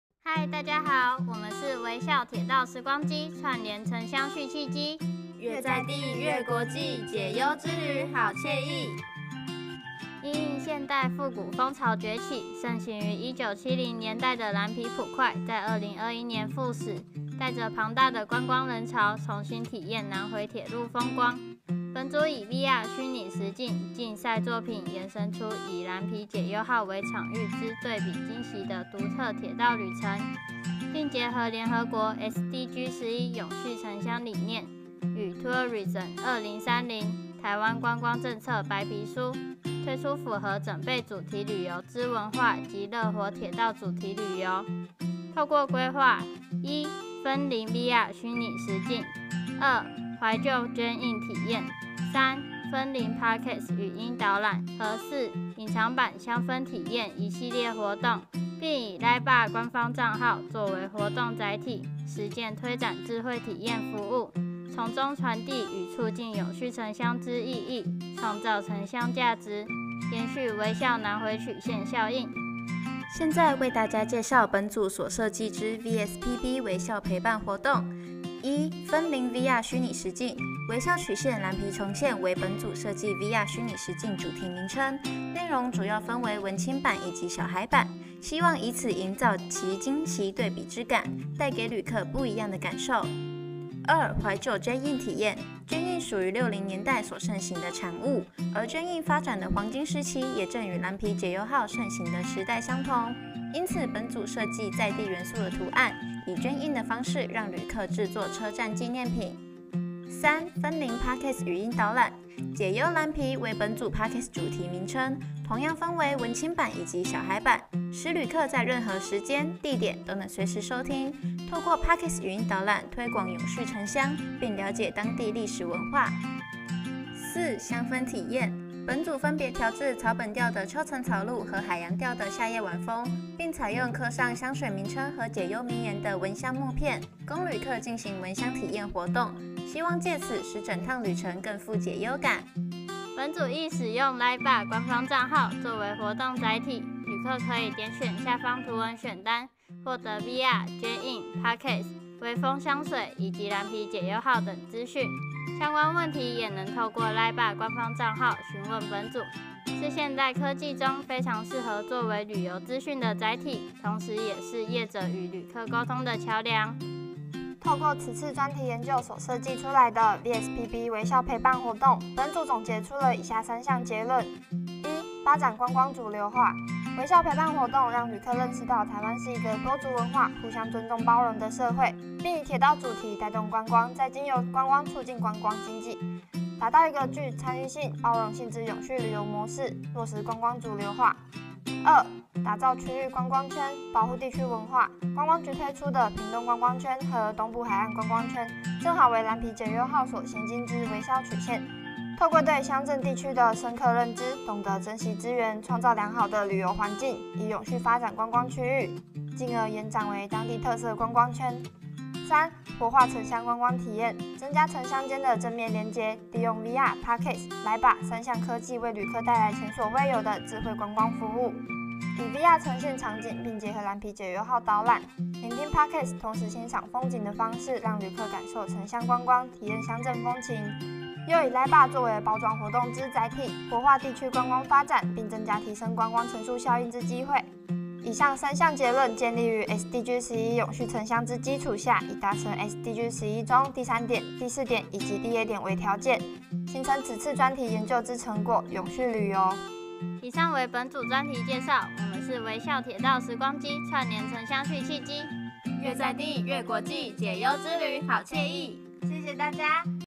112年專題實作及創意競賽決賽【餐旅群】專題組第一名：微笑鐵道時光機，串聯城鄉續契機－越在地越國際．解憂之旅好愜意
得獎獎項：【餐旅群－專題組－第一名】
參賽學校：高雄市立三民高級家事商業職業學校
作品名稱：微笑鐵道時光機，串聯城鄉續契機－越在地越國際．解憂之旅好愜意
指導老師：陳○妏、許○玲
參賽學生：郭○岑(觀光事業科)、詹○紫(觀光事業科)、黃○恩(觀光事業科)